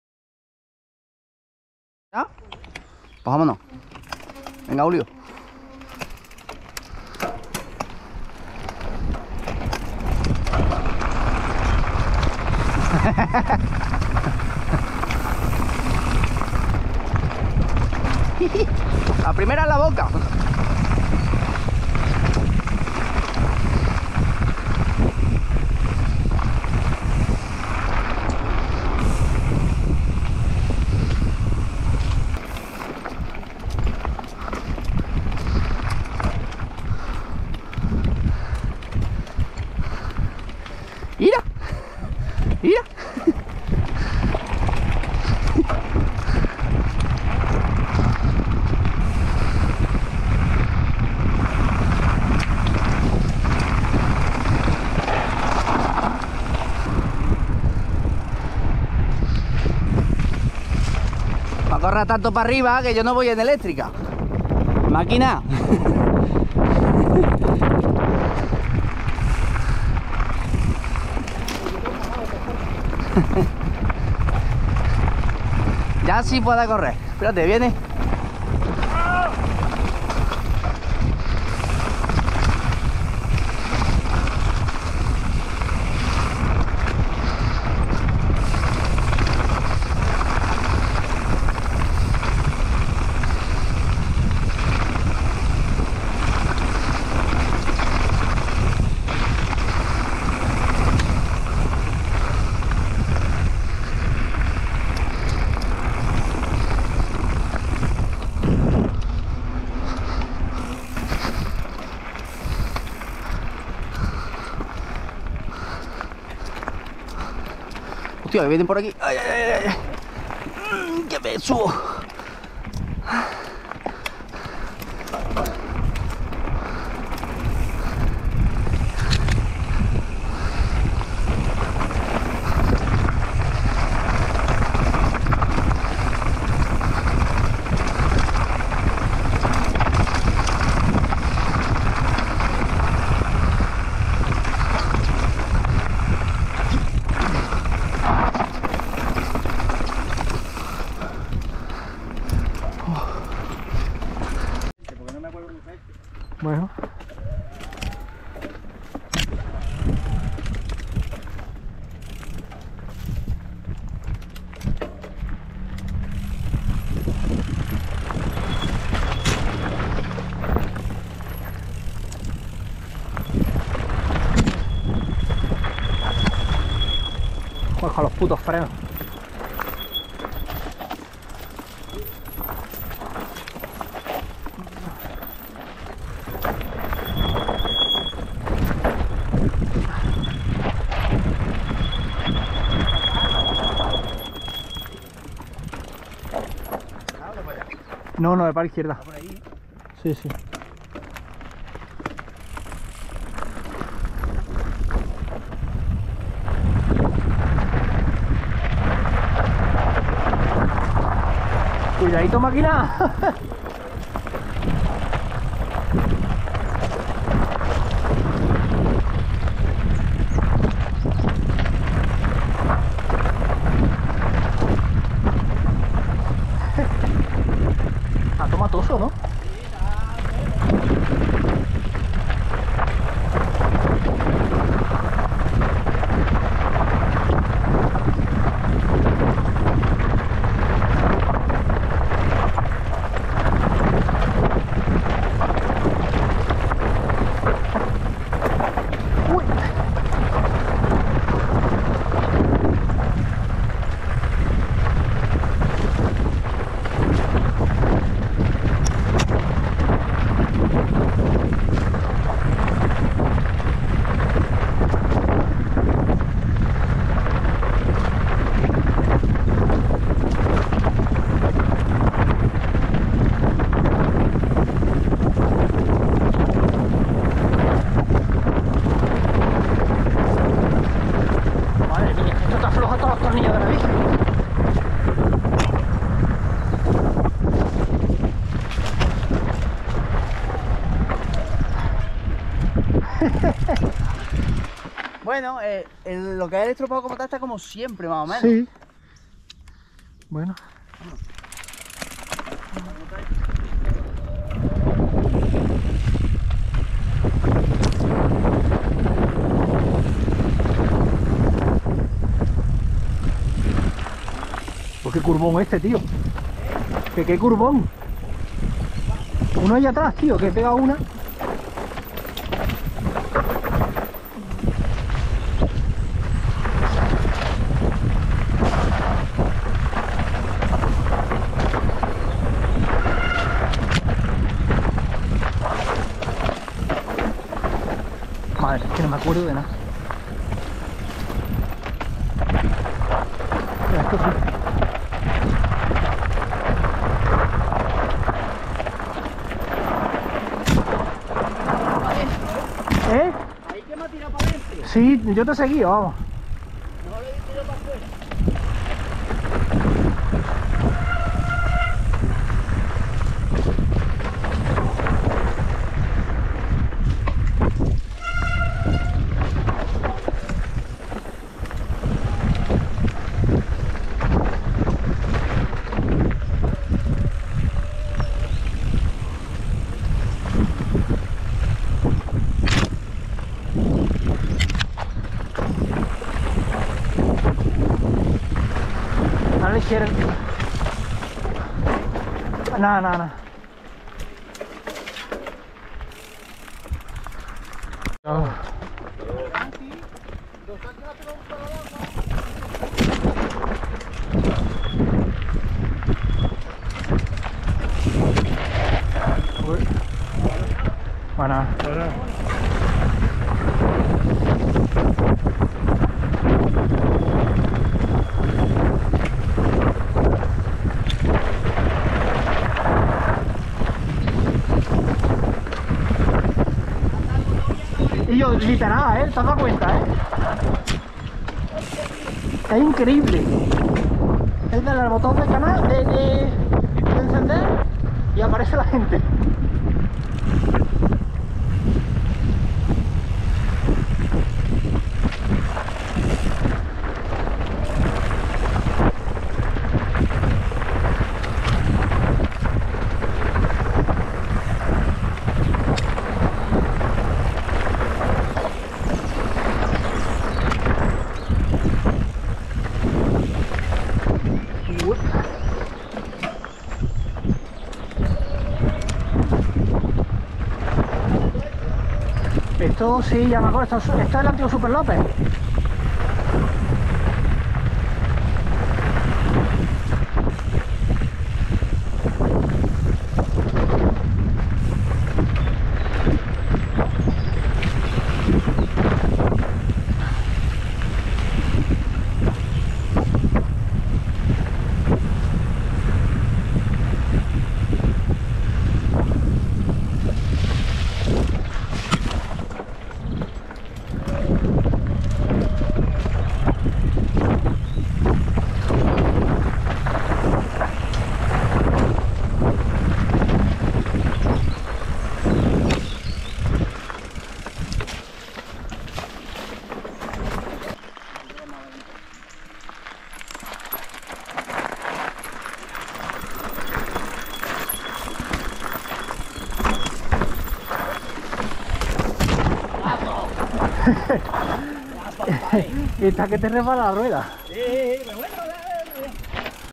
Muchas piedras con David | Estropajo R72 FlowDown | Propain Ekano
Salida de domingo con David y su Transition Patrol. Demasiadas piedras en los descensos por Córdoba.

El vídeo tiene un error, la bajada del minuto 2:44 no es FlowDown, es estropajo + R72, las he intercambiado entre ellas... cosas de la edición.